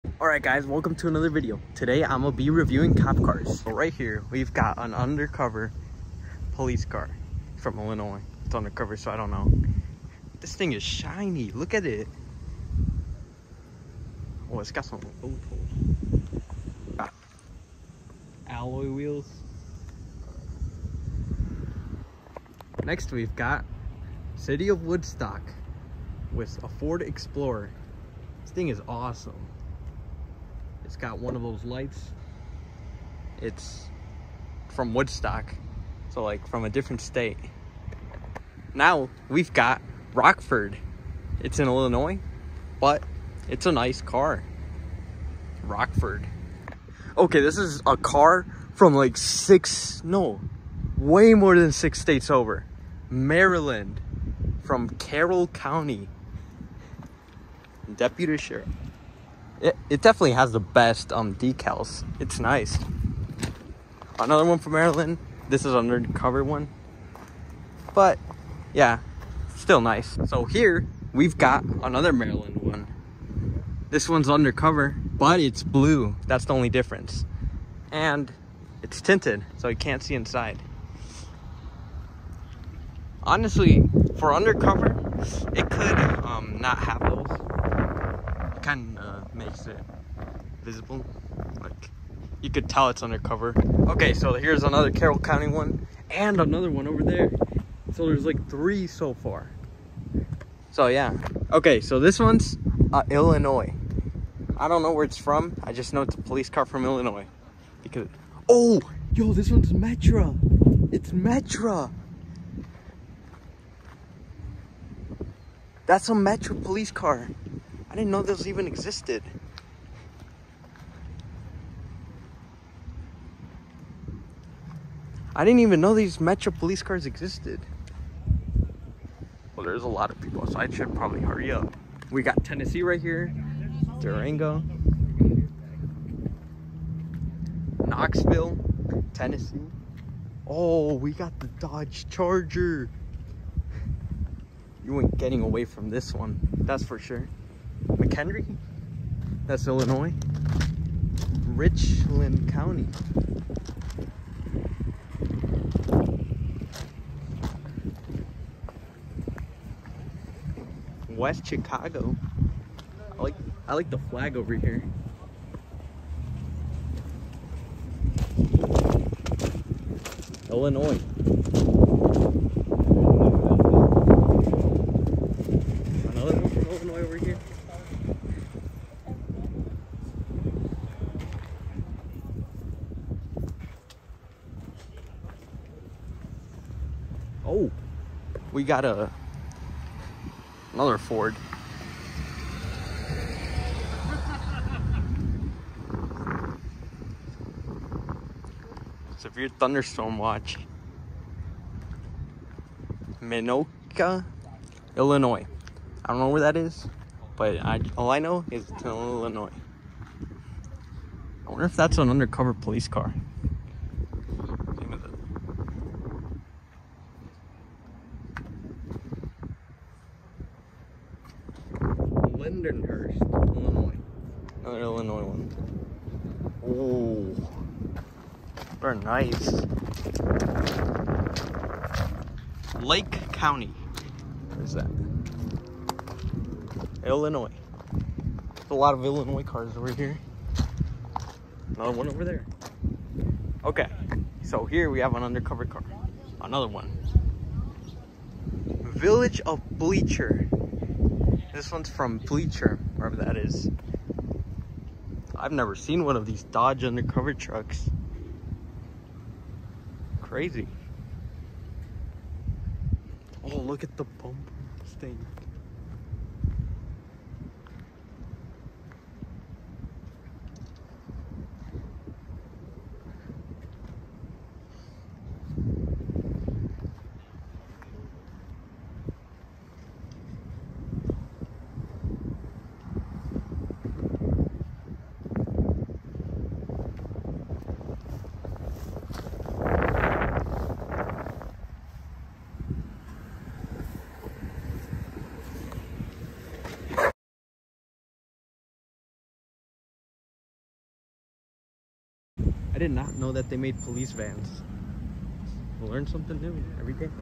Surprising Police Variety in Parking Lot
Watch as police departments from different cities come together in one parking lot for a special event. See the diversity and collaboration among these law enforcement agencies in action!